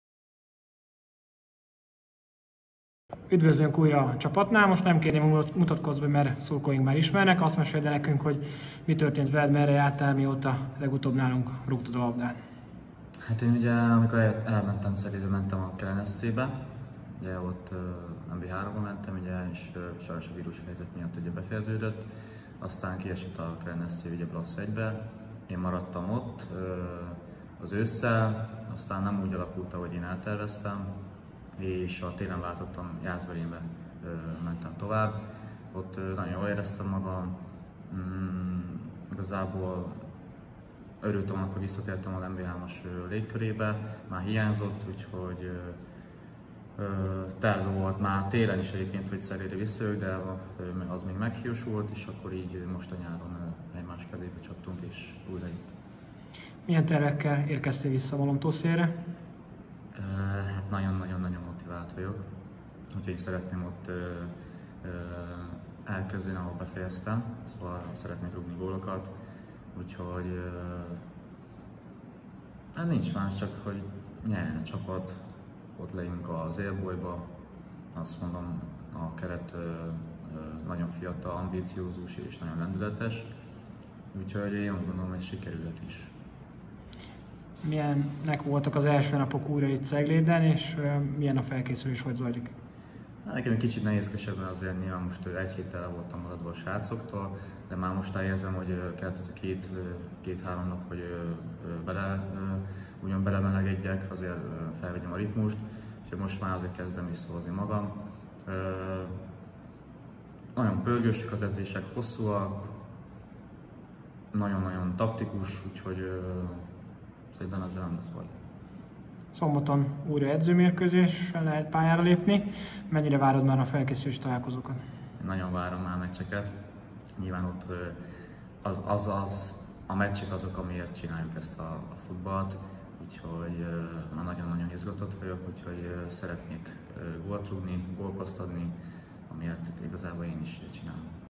Lukács Richárd visszatért
Lukács Ricsivel beszélgettünk visszatérése kapcsán.